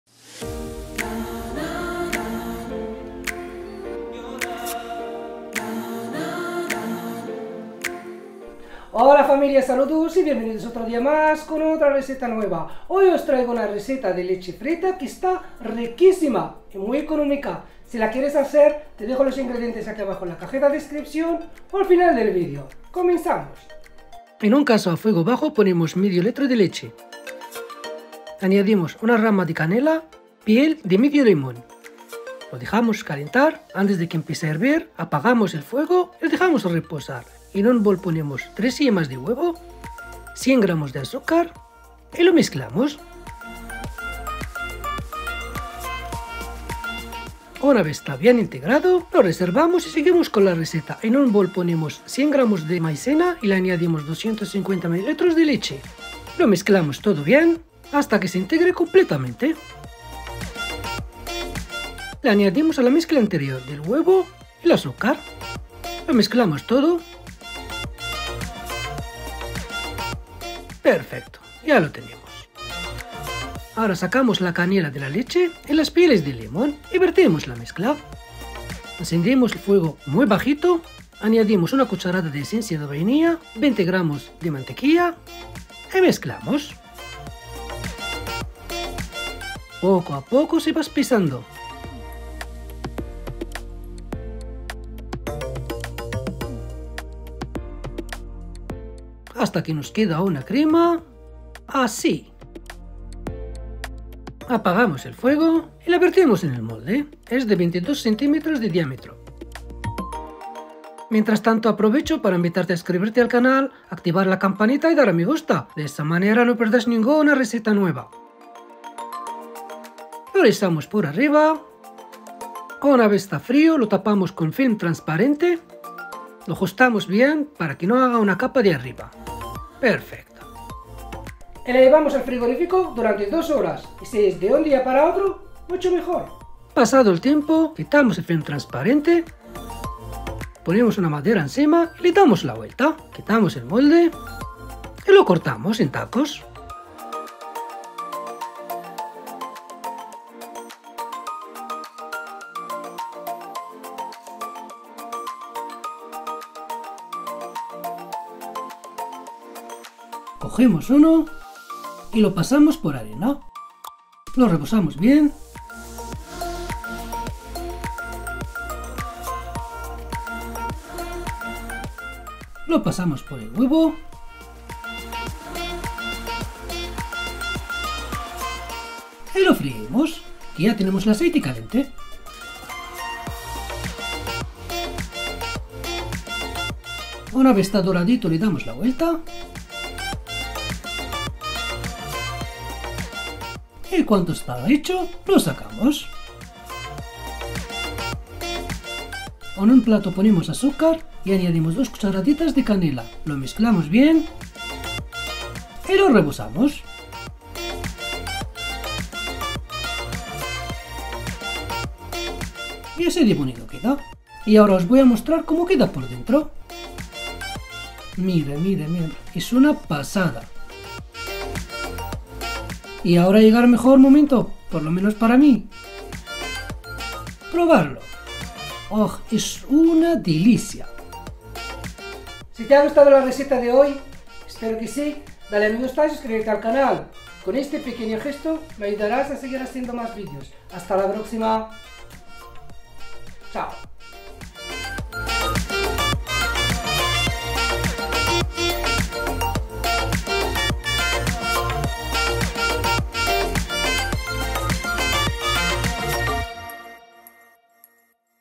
Cremosa LECHE FRITA ¡fácil y rica! | Receta típica española | TAZI OFICIAL
Hoy os traigo la receta de deliciosa leche frita, un postre típico español que está riquísimo y es muy fácil de preparar. ¡El resultado os encantará! Os animo a prepararlo y para cualquier duda os leo en los comentarios.

INGREDIENTES:

500 ml. de Leche
1 Rama de canela
Piel de medio limón
3 Yemas de huevo
100 gr. de Azúcar
250 ml. de Leche
100 gr. de Maicena
1 cda. de Esencia de vainilla
20 gr. de Mantequilla

Refrigerar durante 2 horas y si es de un día para otro, mucho mejor.

Para el rebozado:
Harina
Huevo
Azúcar